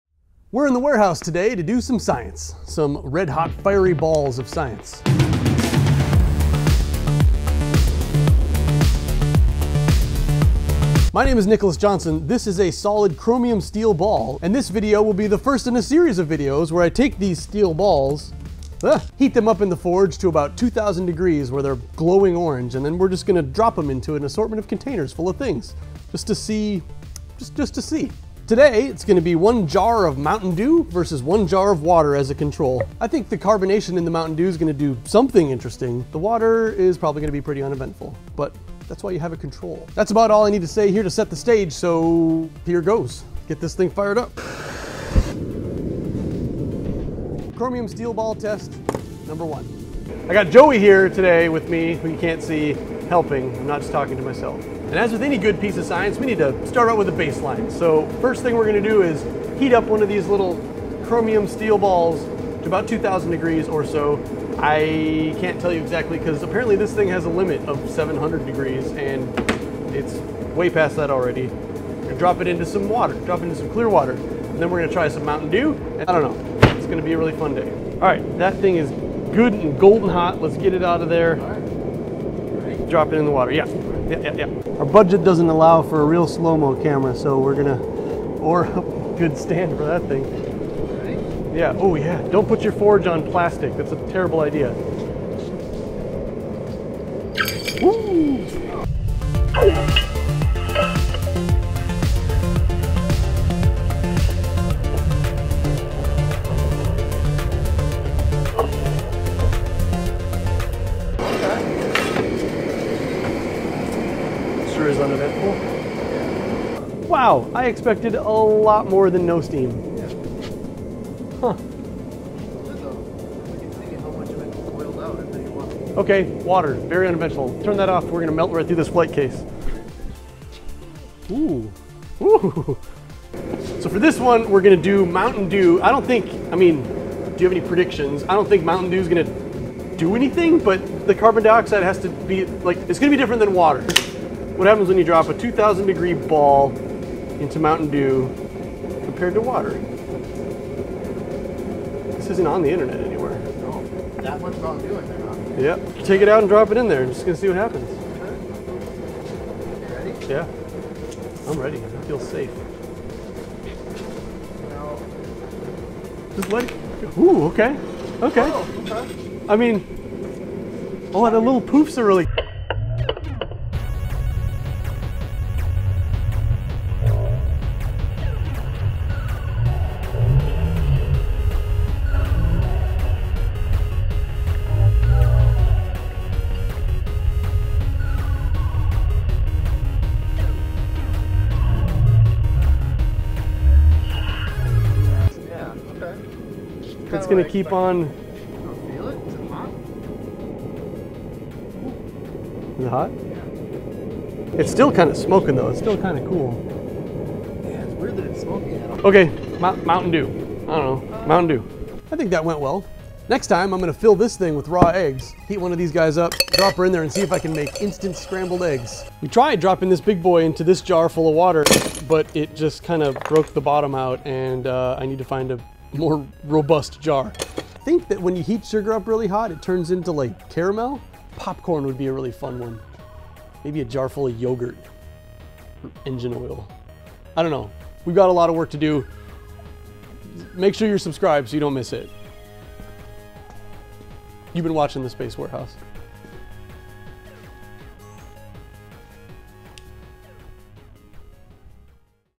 Experiment: Glowing Red Hot 2000 Degree Steel Ball Vs Mountain Dew. (1000 c)
What happens when you drop a glowing red hot metal ball into a jar of mountain dew? Well, we set out to find out!

We are in the warehouse today to do some science!  My name is Nicholas Johnson and this is a solid chromium steel ball, and This video will be the first in a series of videos where I take these steel balls, put them into this propane forge that I built to mess around with some black smithing last year - heat them up to around 2000 degrees where they’re glowing orange and then drop them into an assortment of containers full of different things to see… well, just to see.

Today its one jar of mountain dew and one jar of water as a control.  I figure the carbonization of the mountain dew will do SOMETHING interesting - the water will probably be pretty uneventful.  But thats about all I need to say to set the stage here, so here - goes

Next Im going to fill this shaker with raw eggs, drop in a glowing hot chromium steel ball and see if I can make instant scrambled eggs!  Then we’ll get this big boy involved!! We tried it in water and it shattered the bottom of the container, so I need a bigger jar and I think Ill fill the bottom with sand.. Subscribe to the channel so you see those after I make a couple of improvements so I dont melt through this flight case!

0:00 intro
0:41 fire up the forge
1:20 clear water control test
2:30 the mountain dew test
2:48 plop then slow motion
3:32 up next